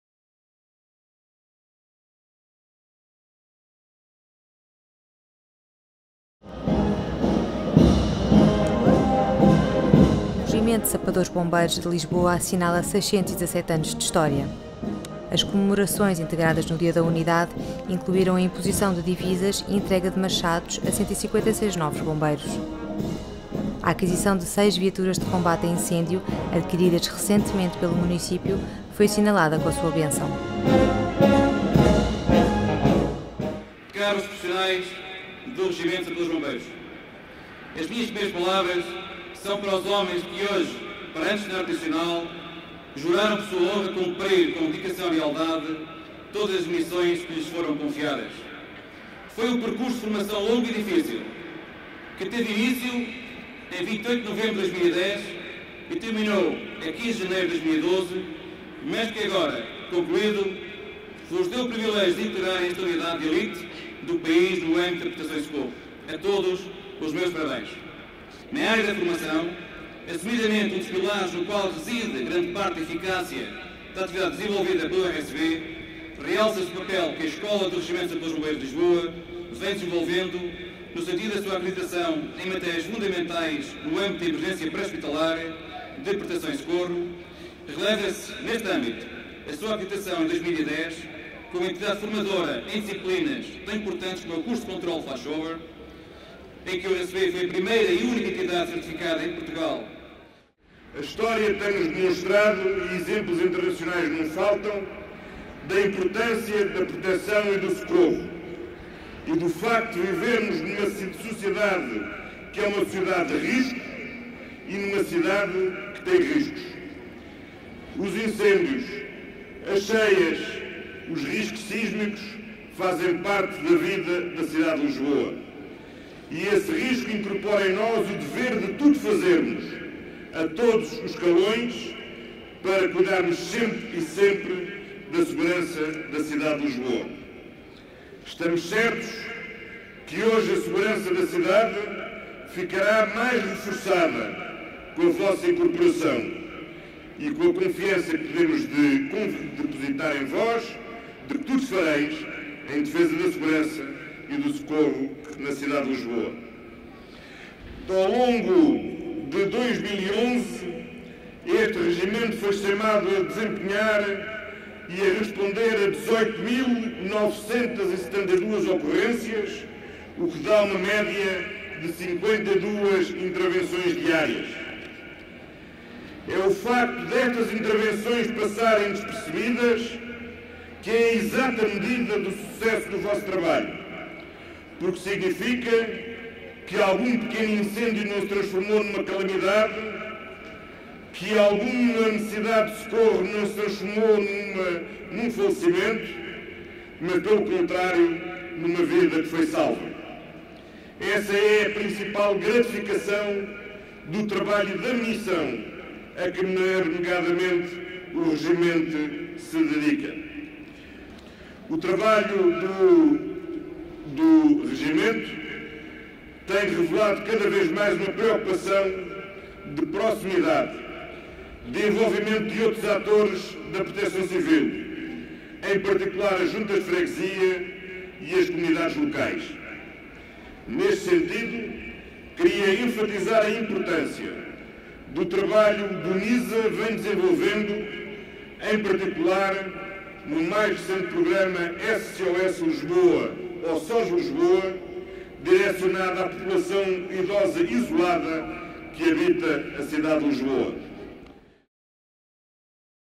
Comemorações dos 617 anos dos Sapadores Bombeiros de Lisboa
As comemorações dos 617 anos dos Sapadores Bombeiros de Lisboa, integradas no Dia da Unidade, decorreram dia 18 de maio, na Praça do Município, com a presença do presidente da autarquia, António Costa